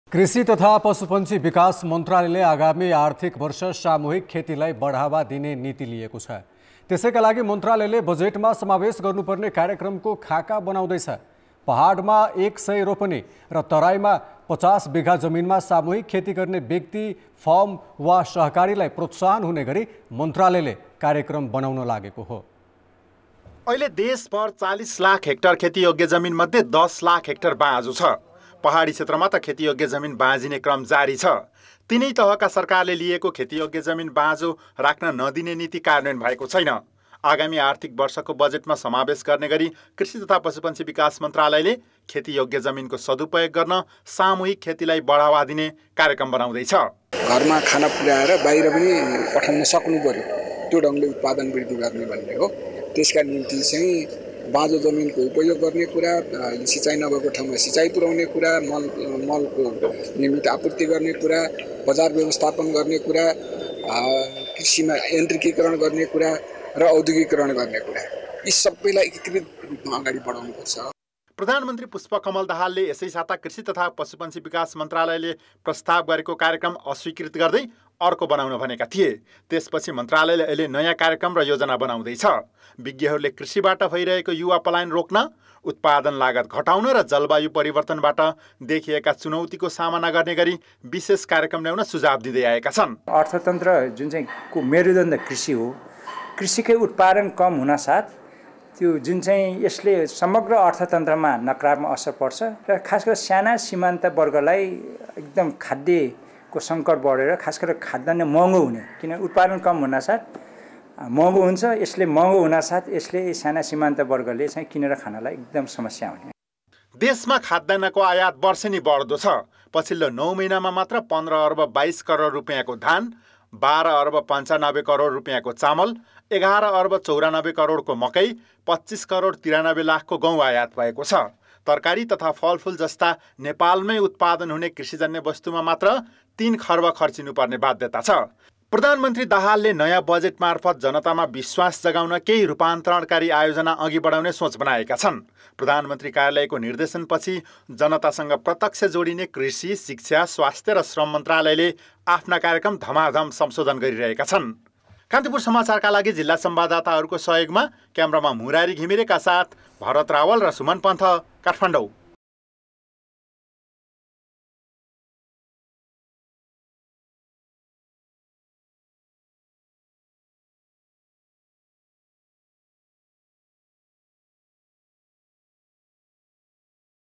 कृषिको प्रस्तावित कार्यक्रम प्रधानमन्त्रीले गरे अस्वीकृत । कान्तिपुर समाचार
कृषि तथा पशुपन्छी विकास मन्त्रालयले आगामी आर्थिक वर्ष सामूहिक खेतीलाई बढावा दिने नीति लिएको छ । त्यसैका लागि मन्त्रालयले बजेटमा समावेश गर्नुपर्ने कार्यक्रमको खाका बनाउँदैछ । पहाडमा एक सय रोपनी र तराईमा ५० विघा जमिनमा सामूहिक खेती गर्ने व्यक्ति, फर्म वा सहकारीलाई प्रोत्साहन हुने गरी मन्त्रालयले कार्यक्रम बनाउन लागेको हो ।